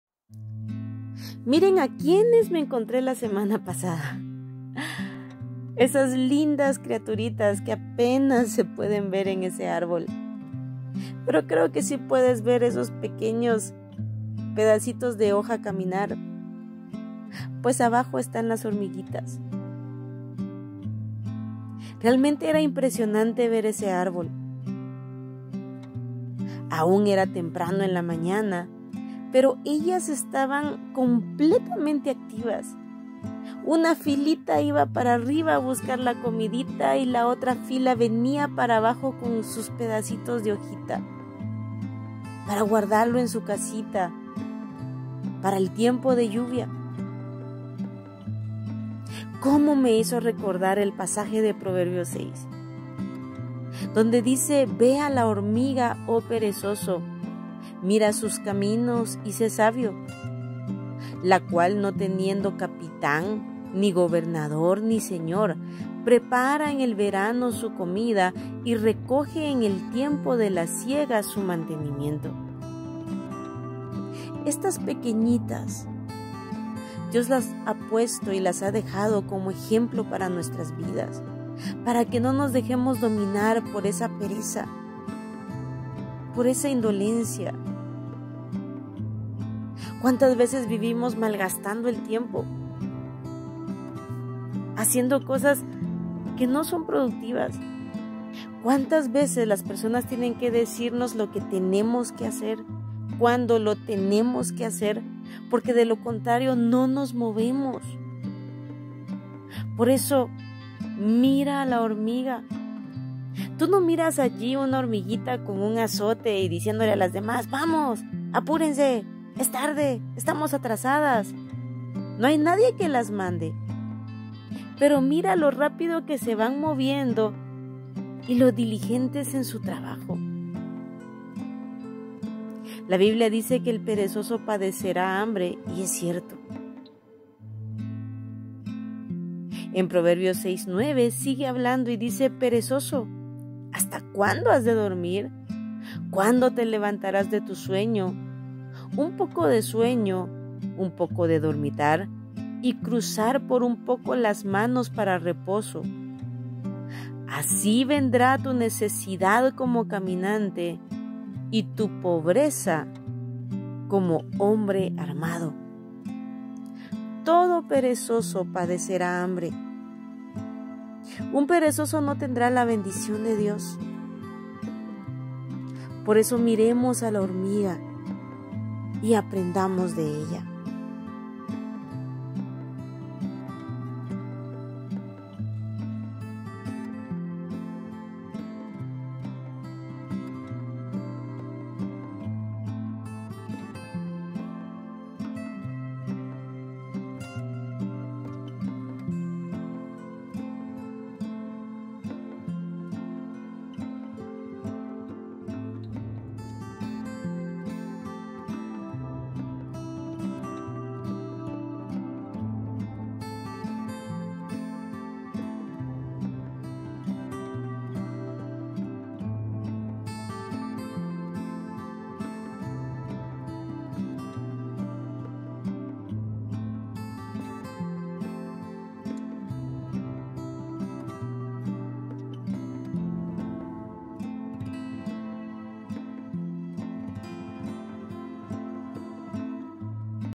REFLEXION CRISTIANA MIRA A LA HORMIGA PEREZOSO, SÉ SABIO!!, EL PEREZOSO DESEA Y NADA ALCANZA
La hormiga es un animalito diminuto pero Dios lo dejó para darnos grandes lecciones de vida😄🙌 ... aprendamos de ellas